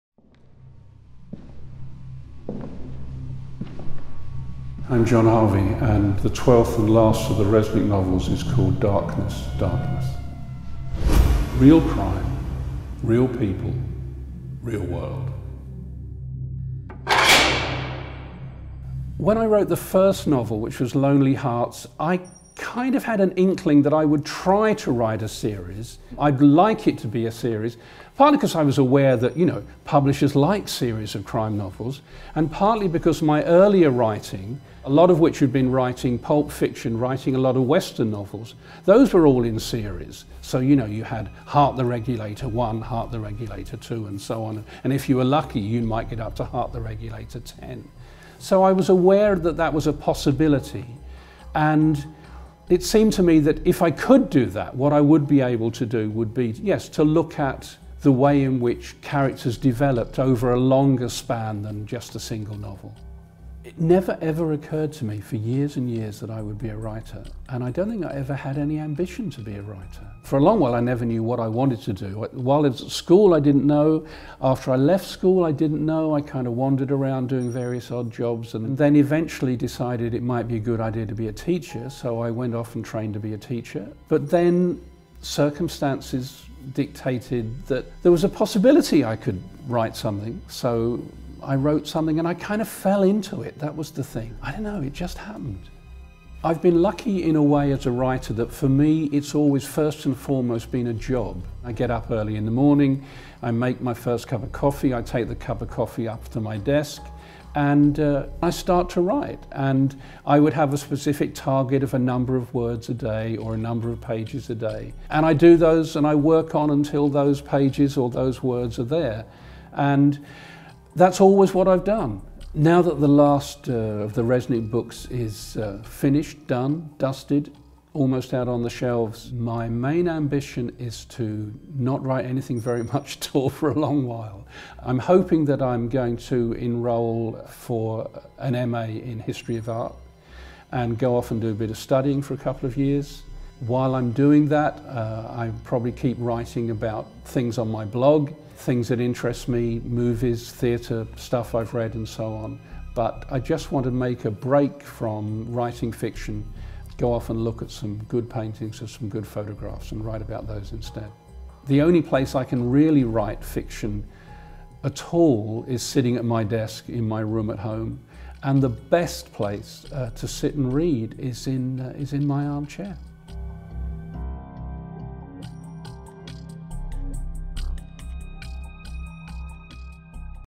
DARKNESS,DARKNESS: John Harvey on his writing career
Bestselling crime writer, John Harvey discusses his 12th and final DI Charlie Resnick novel, Darkness, Darkness and his crime writing career.
Darkness,Darkness is available from 22 May 2014